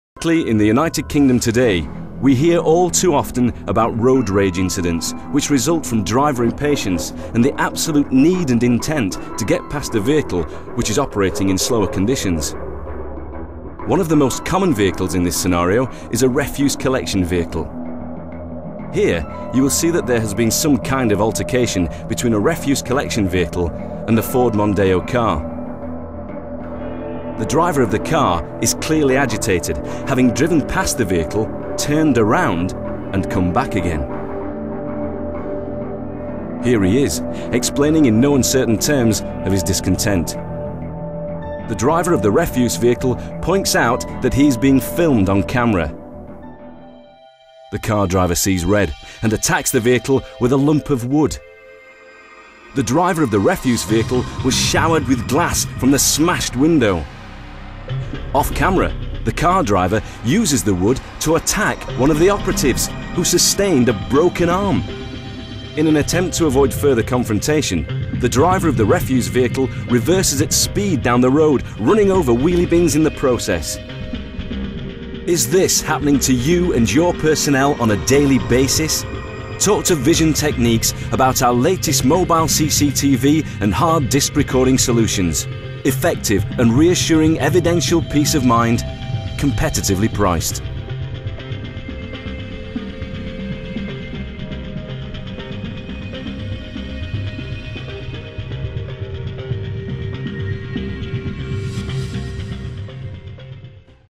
Vision Techniques - Refuse Collection Operative Attacked
Refuse Collection Operative Attacked and caught on Vision Techniques CCTV Hard Disk Recorder